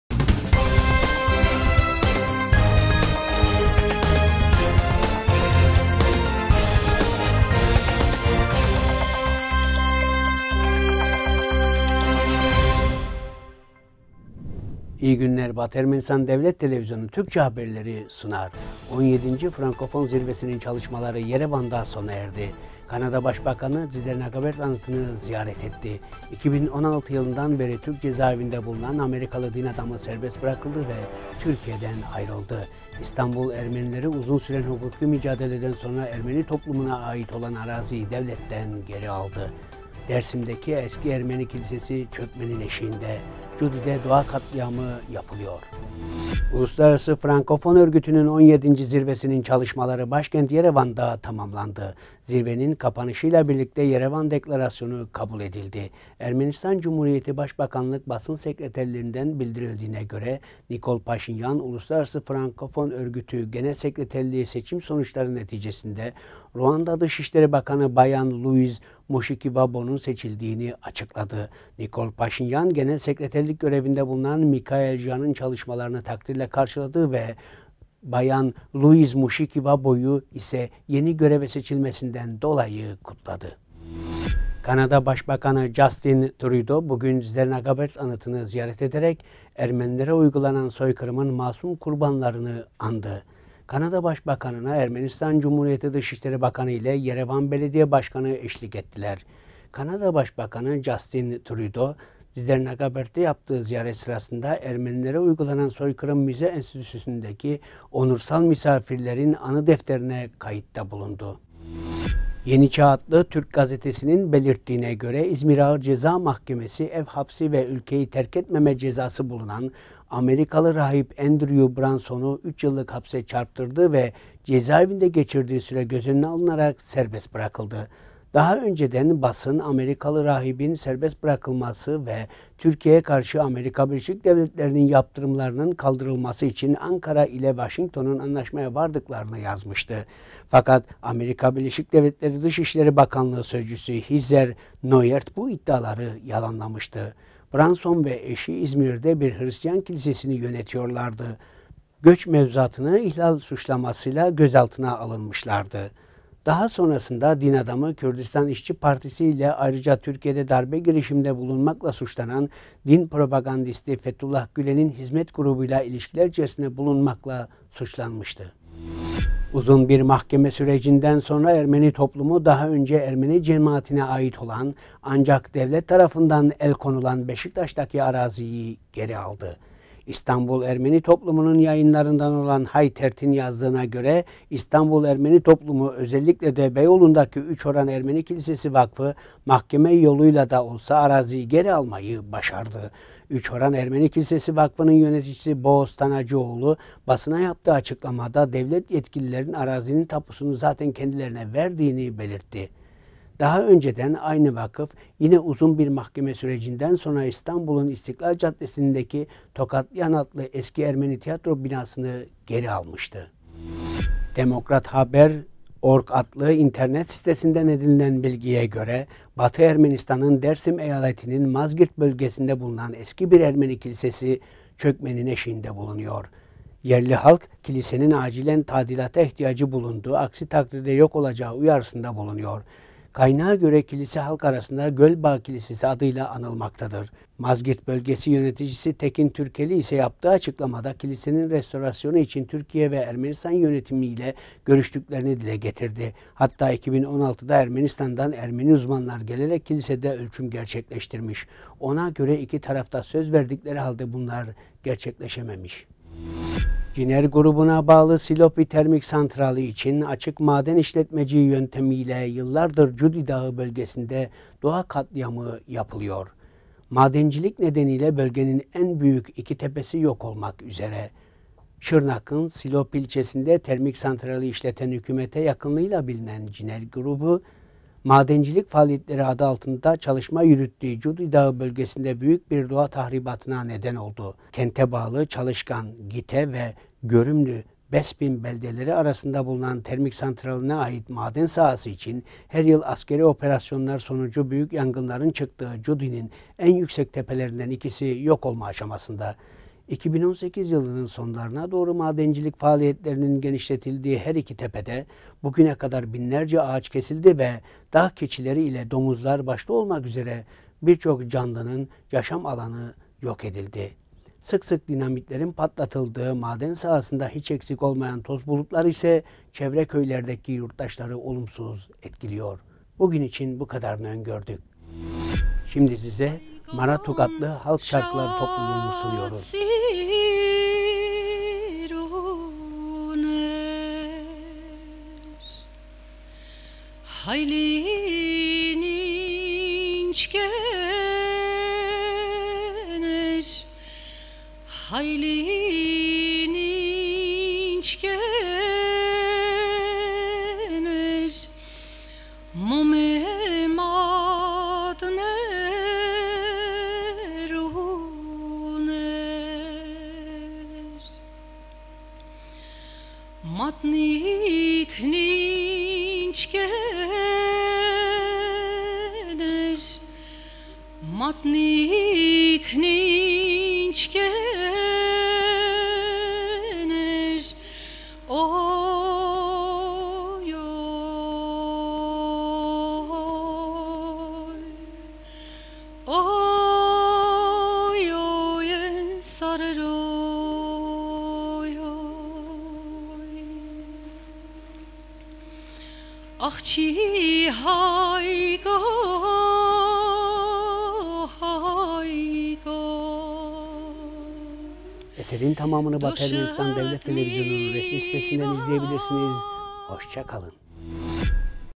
Haberler 2018-10-13
1-  17. Frankofon Zirvesi'nin çalışmaları Yerevan’da sona erdi
2- Kanada Başbakanı Tsitsernakaberd anıtını ziyaret etti
3- 2016 yılından beri Türk cezaevinde bulunan Amerikalı din adamı serbest bırakıldı ve Türkiye'den ayrıldı
4- İstanbul Ermenileri  uzun süren hukuki mücadeleden sonra  Ermeni toplumuna ait olan araziyi devletten geri aldı
5- Dersim'deki eski Ermeni kilisesi çökmenin eşiğinde
6- Cudi’de doğa katliamı yapılıyor